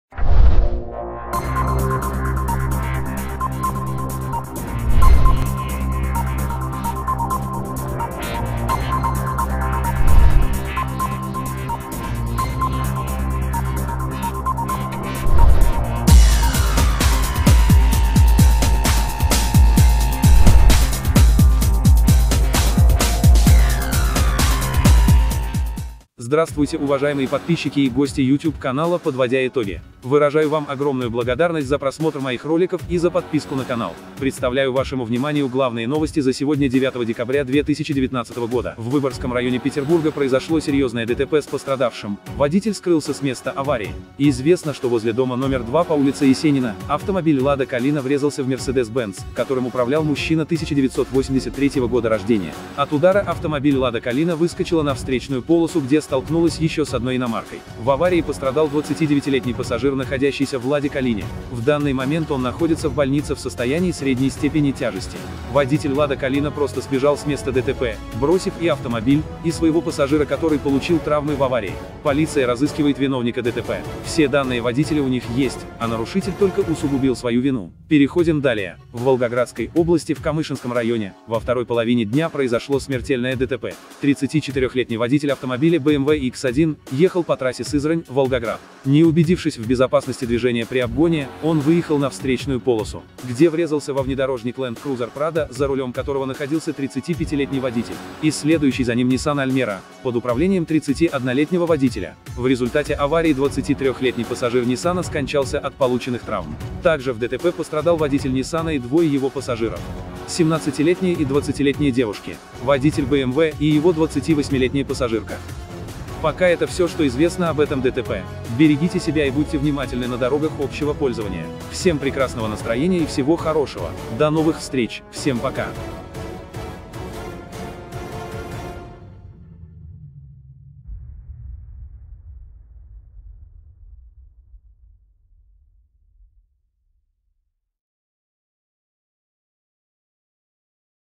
ДТП под Волгоградом один человек погиб и пятеро пострадали. Водитель сбежал с места аварии
Водитель скрылся с места аварии на улице Есенина. В ДТП пострадал 29-летний мужчина
В Выборгском районе Петербурга произошло серьезное ДТП с пострадавшим. Об этом рассказали в пресс-службе регионального управления ГИБДД.
По данным правоохранителей, возле дома №2 по улице Есенина автомобиль «Лада Калина» врезался в «Мерседес Бенц», который управлял мужчина 1983 года рождения. От удара отечественная «легковушка» выскочила на встречную полосу, где столкнулась еще с одной иномаркой.
В аварии пострадал 29-летний пассажир «Лады». Сейчас он находится в больнице в состоянии средней степени тяжести. Все данные на водителя Лады-Калины у правоохранительных органов имеются. На данный момент он находится в розыске.

В Волгоградской области в  Камышинском районе, во 2-ой половине дня  произошло Смертельное ДТП.
34-летний водитель машины «БМВ Х-1» ехал по трассе Сызрань – Саратов – Волгоград в сторону Волгограда. Не убедившись в безопасности движения при обгоне, он выехал на встречную полосу, где врезался во внедорожник «Ленд Крузер Прадо» ,за рулем которого находился 35-летний водитель, и следующий за ним «Ниссан Альмера», под управлением 31-летнего водителя. В результате аварии 23-летний пассажир «Ниссана» скончался от полученных травм. Также в ДТП пострадал водитель «Ниссана» и двое его пассажиров – 17-летняя и 20-летняя девушки, водитель БМВ и его 28-летняя пассажирка.

Мы продолжаем следить за событиями и держать вас в курсе событий